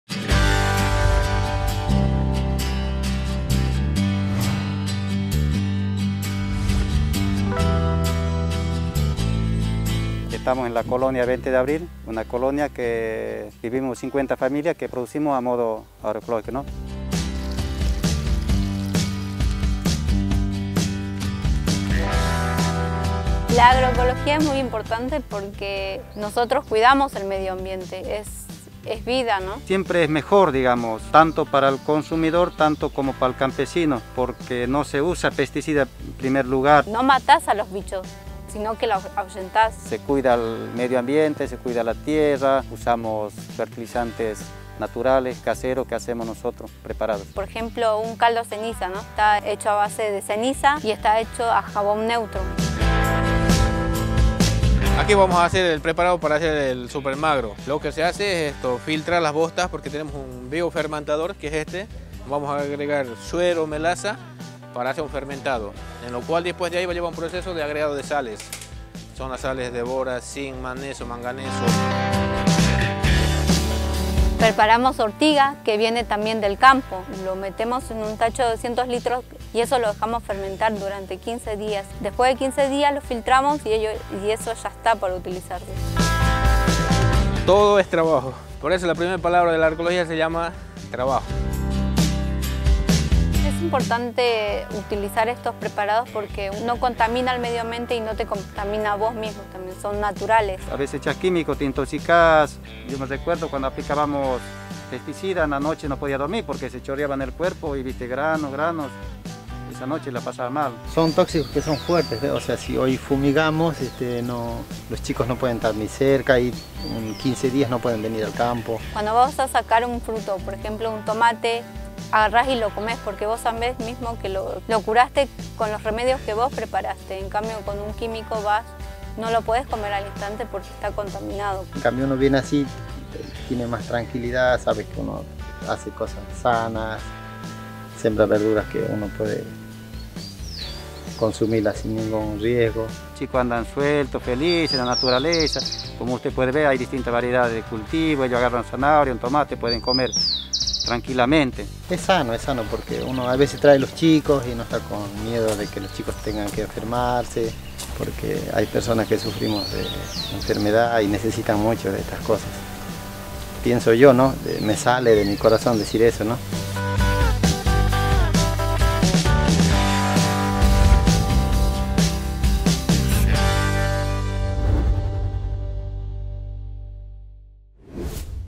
¿Cómo proteger los cultivos y el medio ambiente? - Greenpeace Argentina
En la colonia 20 de abril de la UTT viven 50 familias que producen de manera agroecológica, cuidando al consumidor, al campesino y al medio ambiente ❤️

Te contamos cómo hacen para cuidar los cultivos sin usar químicos peligrosos.

Acá podés descargar el calendario estacional y la guía de cultivo de tomates, para que puedas empezar a cambiar tu alimentación